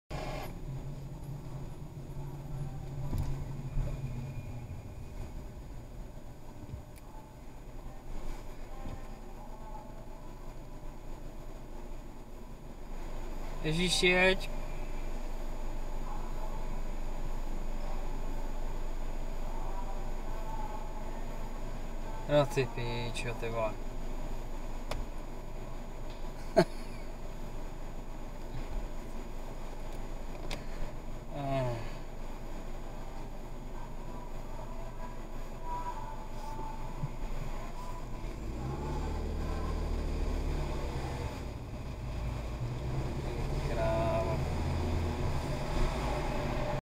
Mistr parkovač [Lyrick za volantem 02]
S01E02: Aneb jak necouvat

INFO: Video z budoucí pravidelné série "Lyrick za volantem"
Řidičem jsem od roku 2005, poslední roky nájezdem 40.000km.
Považuju se za zkušeného řidiče, navíc jezdím defenzivně. Co se nám narodil prďola, tak to řeším extra. Tohle bude série o tom, co mě překvapí, nasere,...

Pro mé gaming friends: Sorry jako, tenhle kanál už nikdy nebude jako dřív.